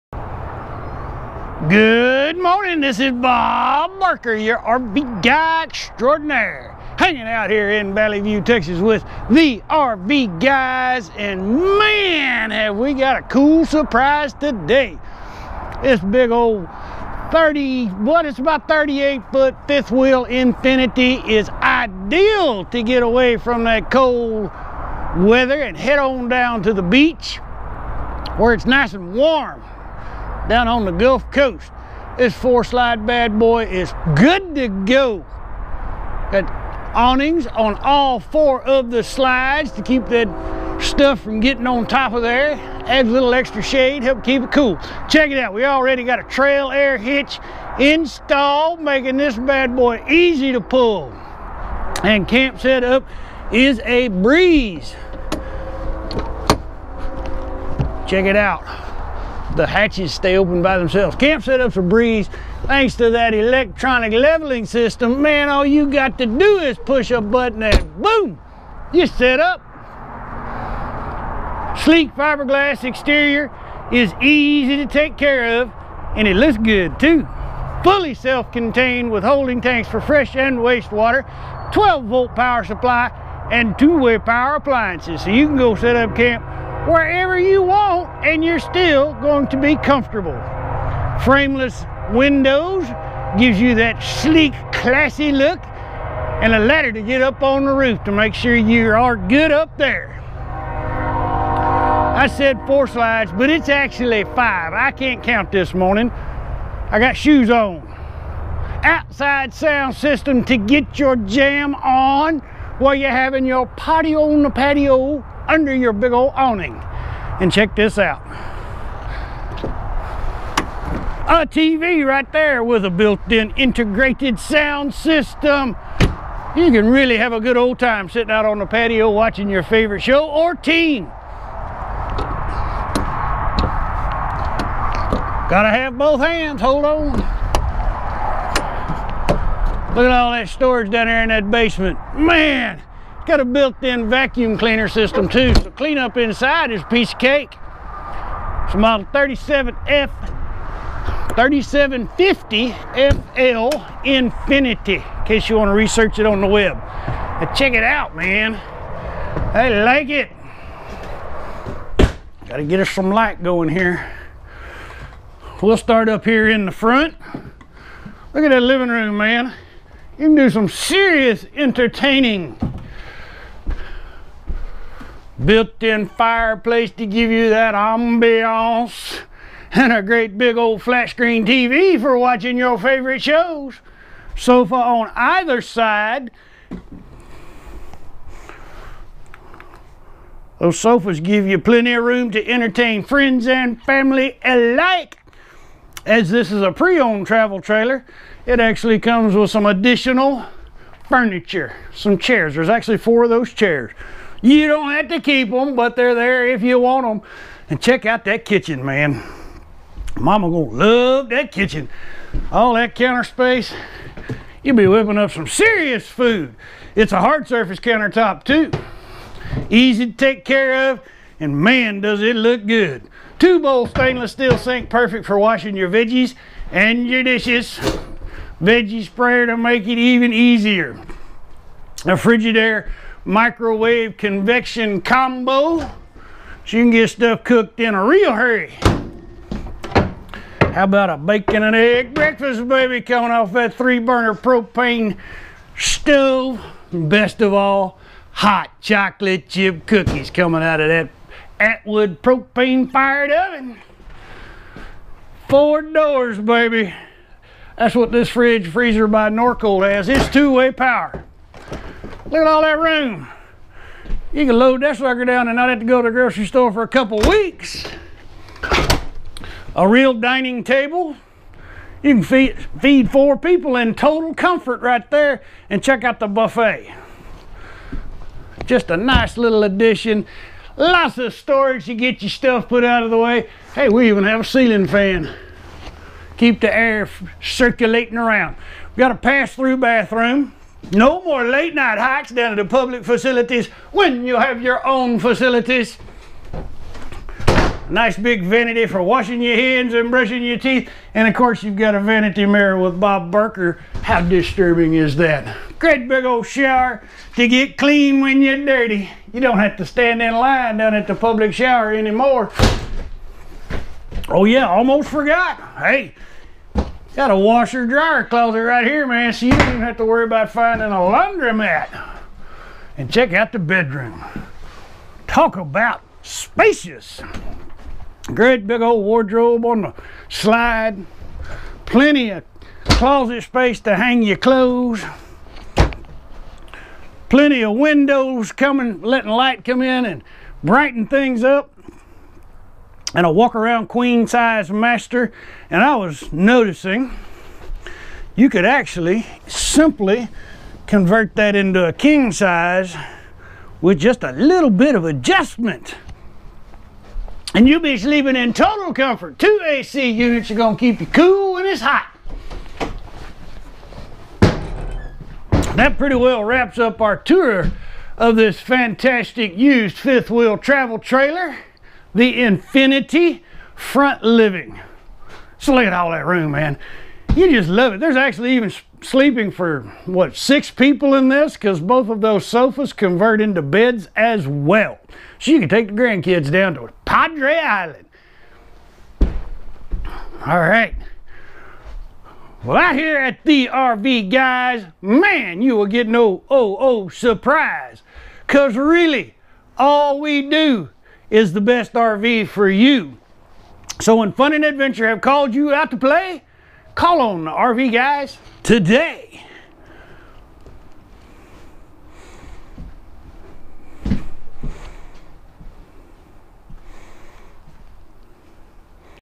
Tons O Room In This Fantastic Front Living Full Timer 5th Wheel! 2013 Infinity 3750FL
Wild and far away places are calling you out to play! This exciting front living fifth wheel is just what you need to get out get into some real fun and excitement! Your spacious front living is just perfect for entertaining friends and family! Boondocking and Dry camping is now done in style and comfort thanks to being fully self contained with holding tanks for fresh/waste water, 12 volt power supply and two way power appliances! Now that you have sleeping for up to 6 your grand kids can get in on the fun too!
 Imagine relaxing by a roaring camp fire way out in the woods, spending the day catching lots of big fish from a remote mountain stream or how about relaxing by a warm romantic fire while eating smors with the one that you love! Many wonderful gourmet meals will be created in your very well appointed kitchen that features plenty of storage for all of your cooking supplies! A spacious full bath with a roomy hot shower will save you from late night hikes down to the dirty and over crowded public facilities! Just imagine all of the many wonderful places you will visit and great memories that you will create while traveling and exploring far away places!  After all of that fun you will rest in total comfort thanks to the oh so cozy queen master bed and super cold A/C unit.
Now is the season for fun, excitement, travel and adventure! Dont wait! Tomorrow never comes and you can not turn back The clock! Your new life of adventure is waiting for you right here in this exciting ful timer friendly fifth wheel travel trailer! Adventure and fun have called you out to play!